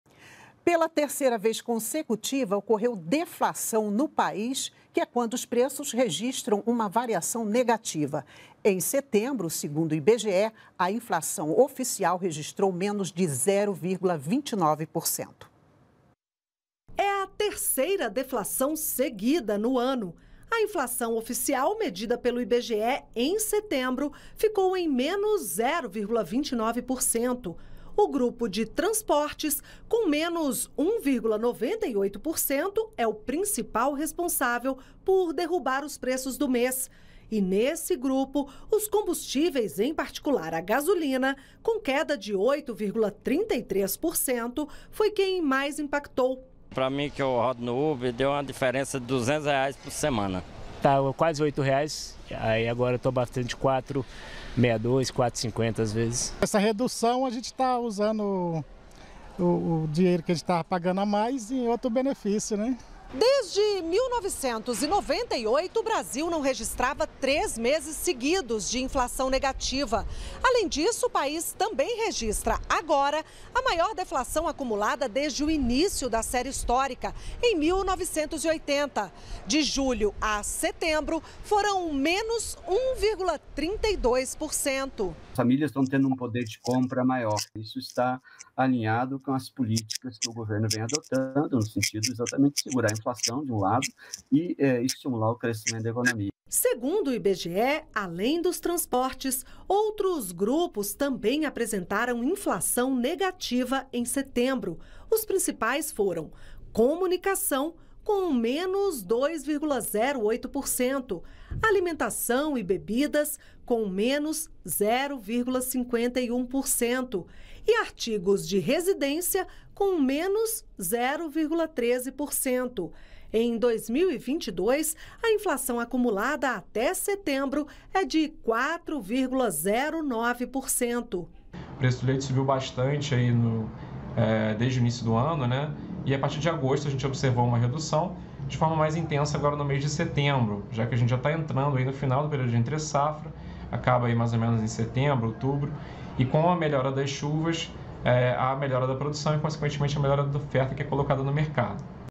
Economia | Deflação em setembro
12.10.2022 - Pela terceira vez consecutiva, ocorreu deflação no país, ou seja, houve queda nos preços. Em setembro, segundo o IBGE, a inflação oficial registrou menos 0,29%.